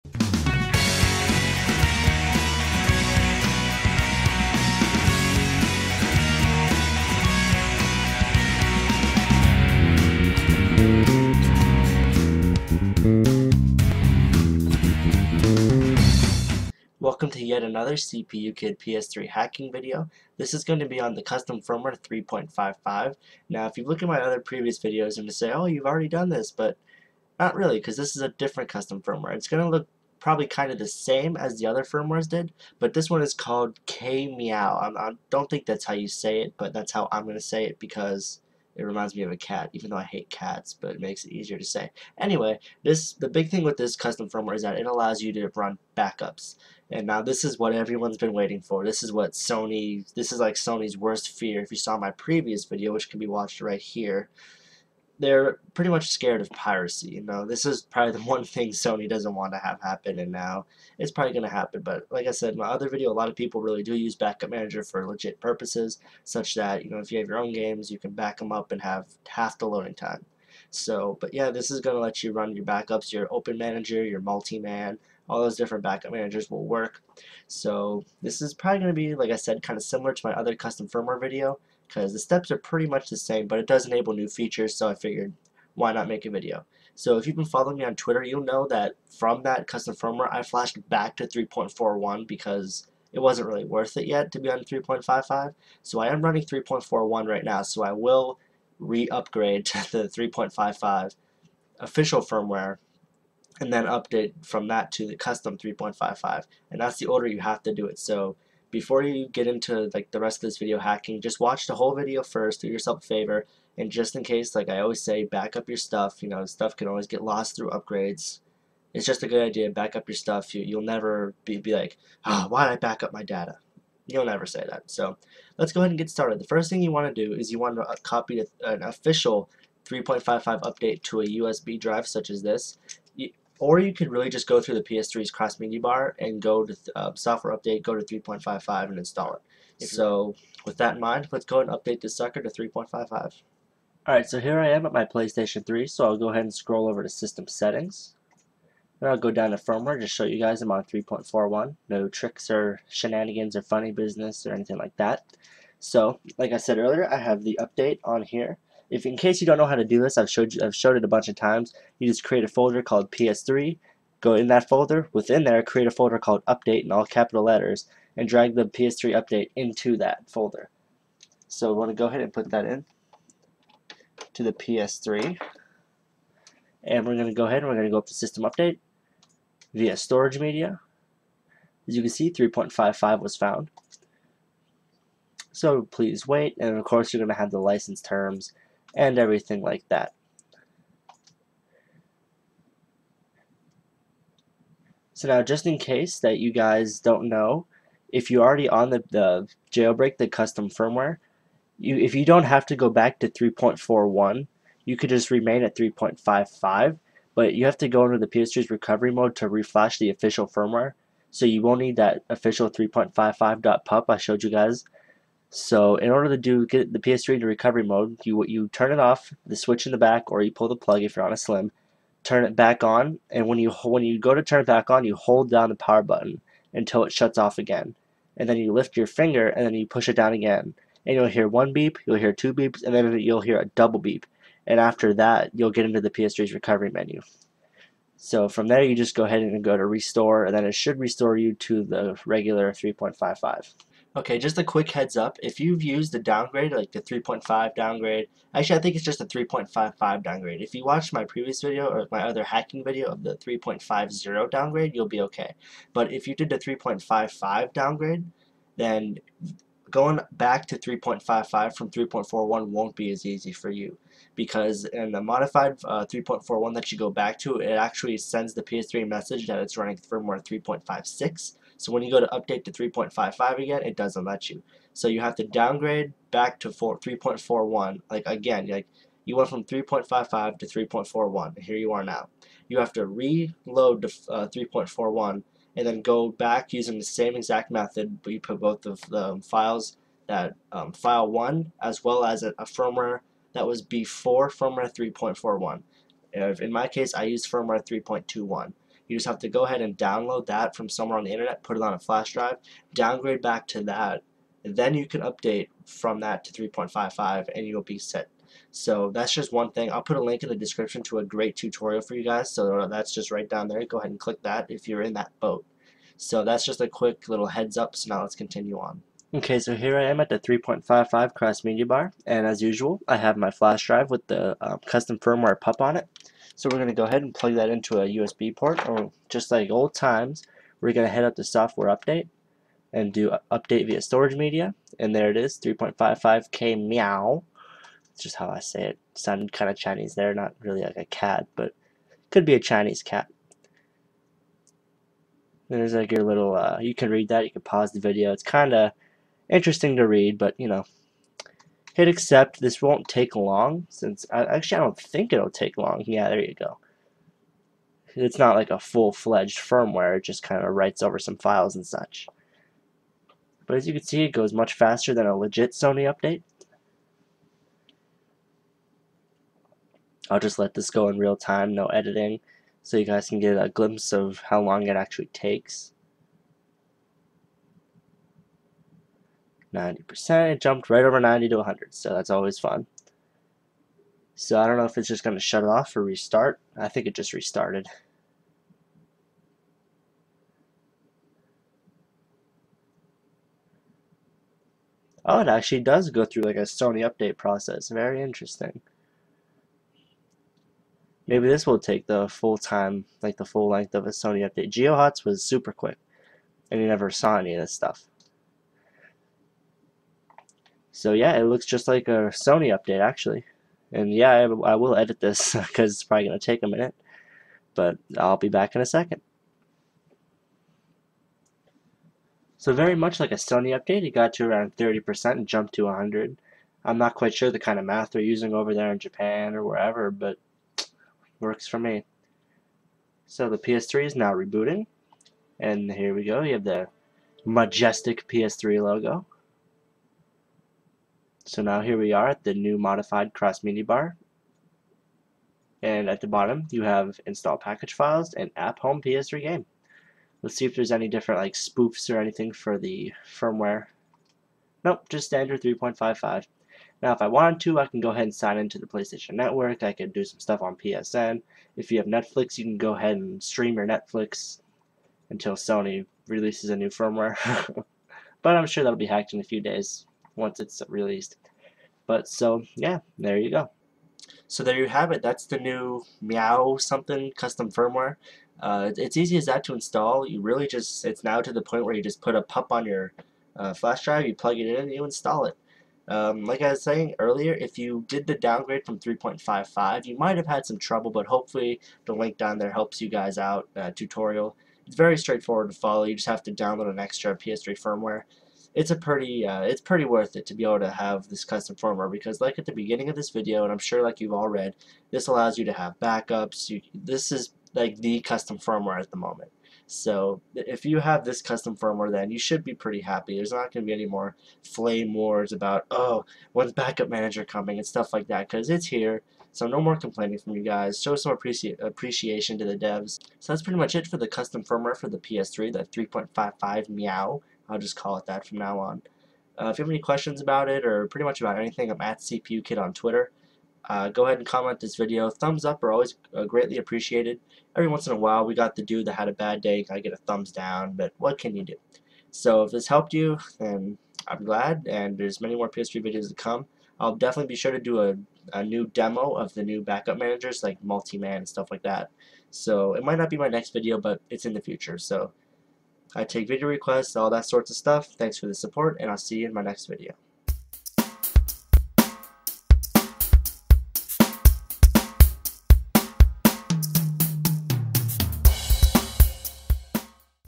How To: Install kmeaw's 3.55 CFW [PS3]
This is a video on how to install the newly released kmeaw 3.55 custom firmware. This custom firmware allows backups to be played, as long as you have the lv2.pkg file (Link below)

Link to the patched PUP (The actual kmeaw firmware file and lv2 patches):

*Use like my previous 3.55 Downgrade video!